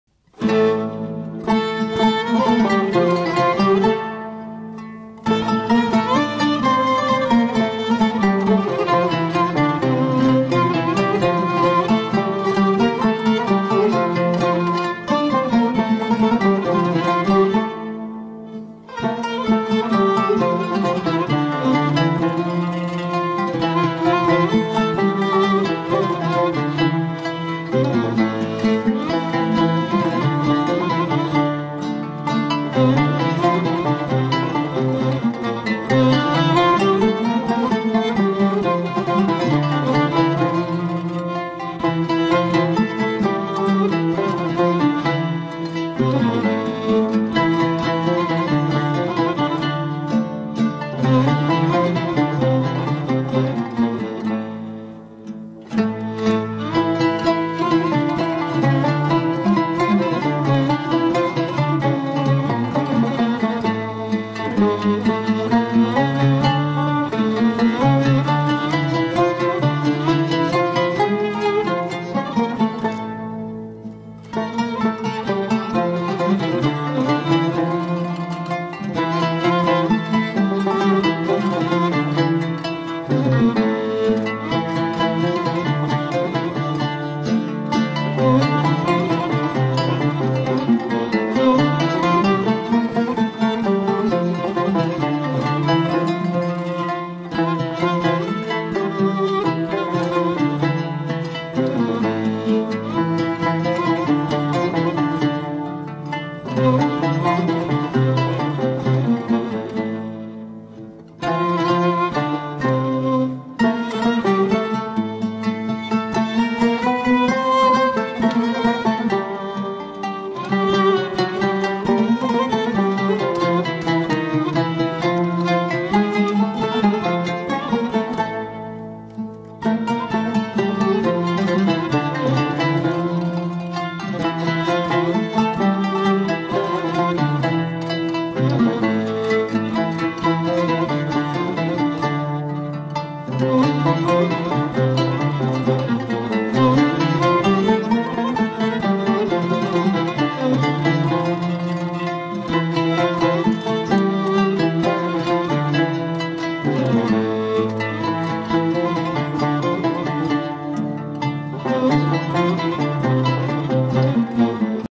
Ebru Art by Sevim Surucu- Rose - Gül ebrusu
Ebru artist Sevim Surucu
Ebru is a traditional Turkish paper marbling art of creating colorful patterns and images by sprinkling color pigments on water, then transferring them to paper. The tools are horsehair brushes, metal pins for shaping paints, specially prepared earth pigments and a tray full of water thickened with seaweed powder. The authenticity of the materials creates a mystique that will take you into the 17th century. Colors that we will be using in the workshop are also from the natural colors of masters' palettes.